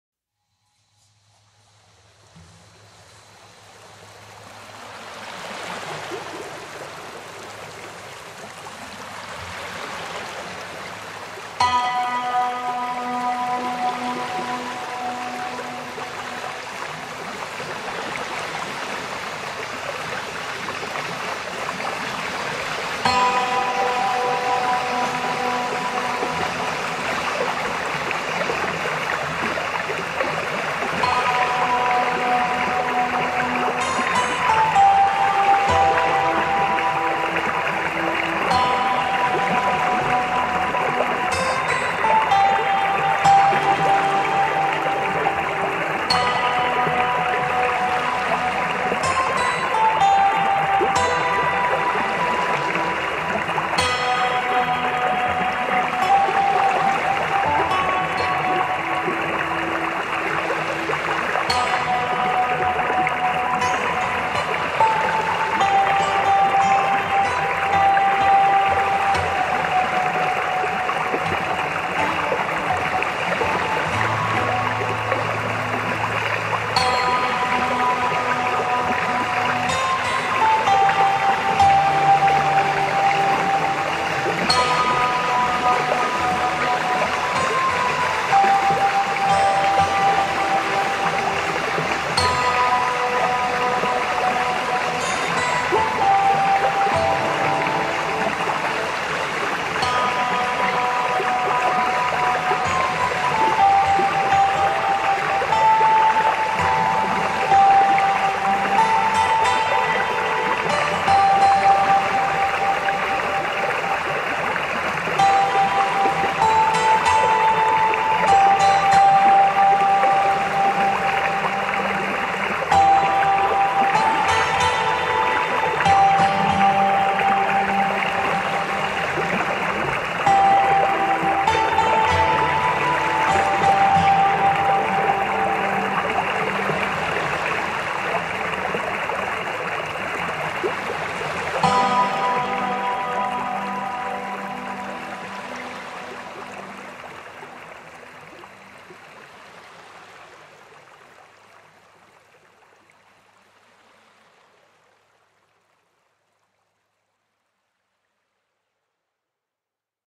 Рассвет над альтернативным океаном
Футаж для видеомонтажа начала создаваемых Вами видеороликов и фильмов: Рассвет над альтернативным океаном. Великолепная и очень качественная графика. Оригинальное музыкальное сопровождение.

Хотите обсудить мои работы и пообщаться с автором? Жду Вас в комментариях!